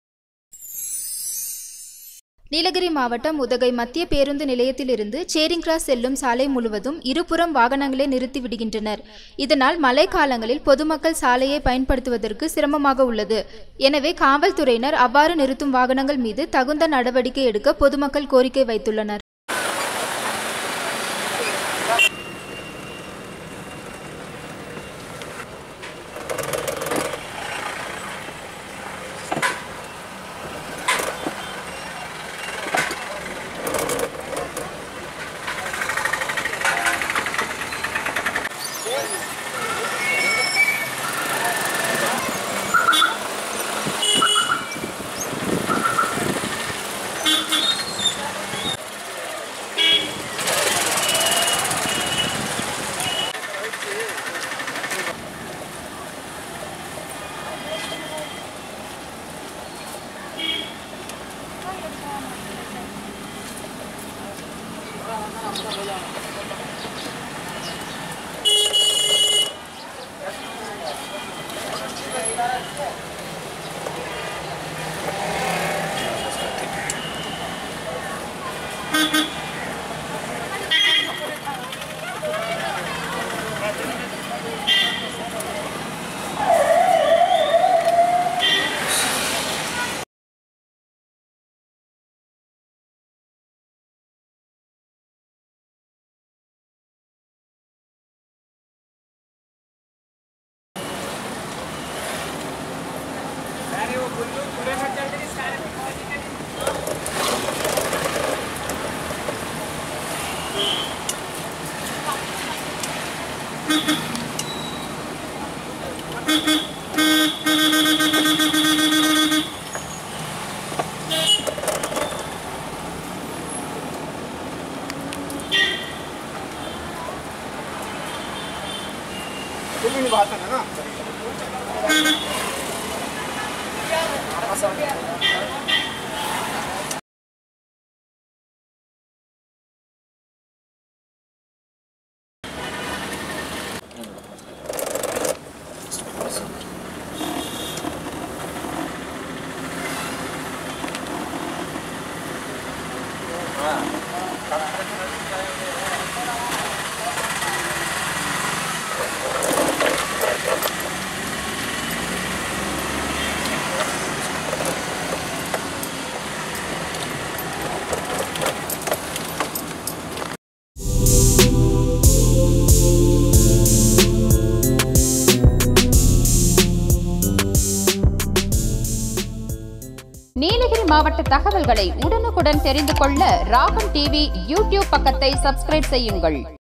பொதுமக்கள் சாலையை பயன்படுத்துவதற்கு சிரமாக உள்ளது | RAGAMNEWS
இருபுறமும் வாகனங்களை நிறுத்துவதால் பொதுமக்கள் சாலையை பயன்படுத்துவதற்கு சிரமாக உள்ளது